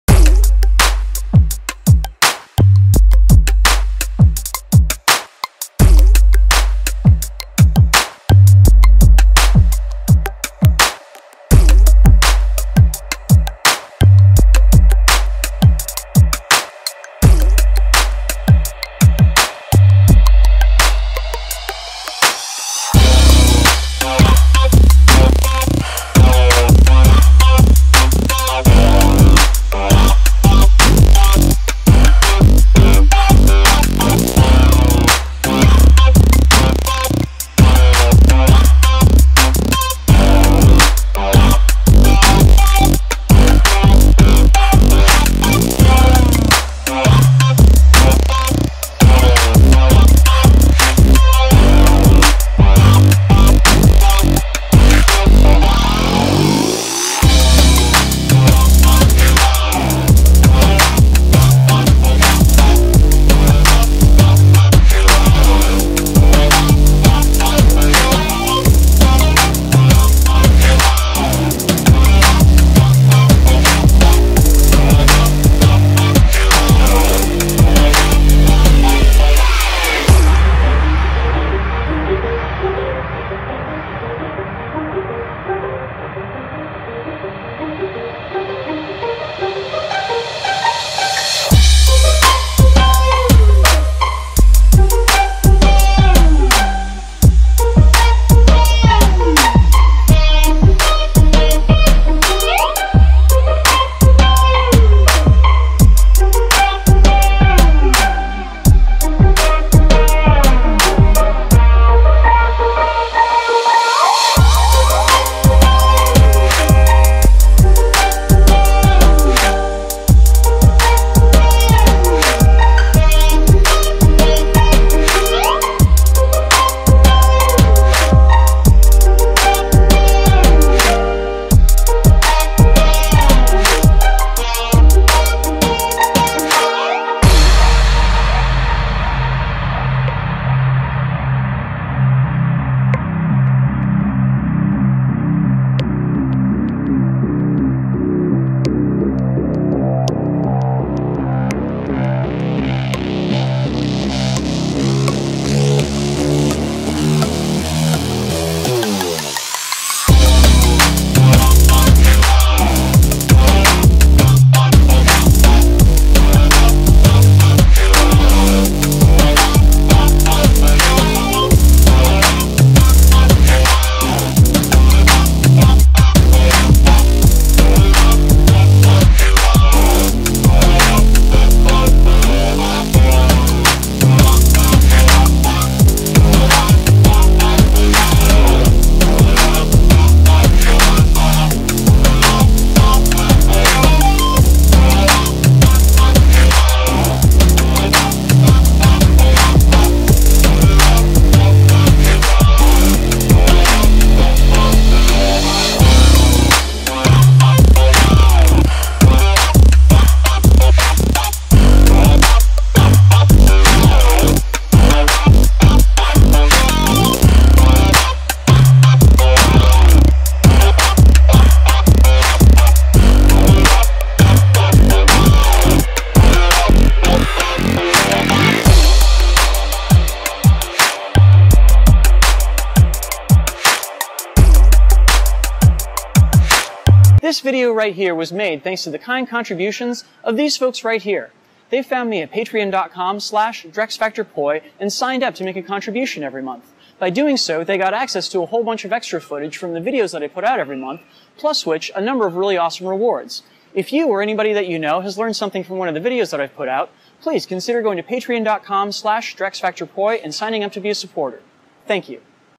Double Staff Grid Tracing Simulations
I’m on a simulation kick! Here are simulations for all the grid tracing patterns that Casey and I did as tunnels in our video last week These include 3 standard grid tracers with hands in together same or split opposite timings and one antibrid tracer that you can find a tutorial

Nick Krohn
EJ Frost
Dominic Lamatrice
Knut L
Matt Schmidt
Jonathan Maltz